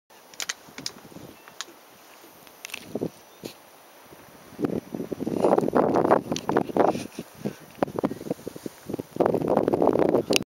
Crane Beach Boardwalk part 2
Part two short clips of Crane Estate Beach.
Ipswich, Ma.
Another nice day, love walking the ocean beach for hours.